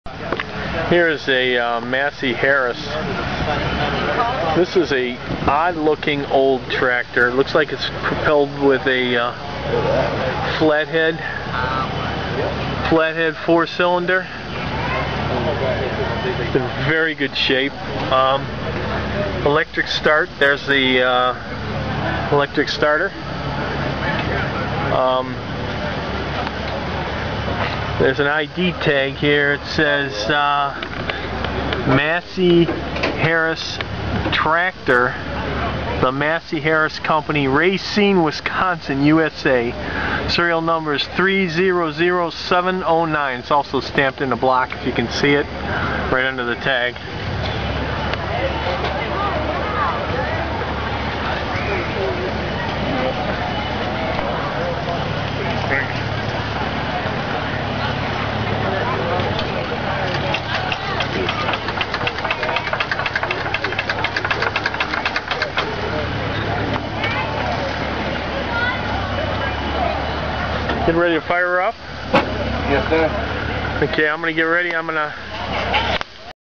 Clean Vintage Massey Harris Tractor
My favorite oil treatment for old engines
Vintage Massey Harris Tractor Walkaround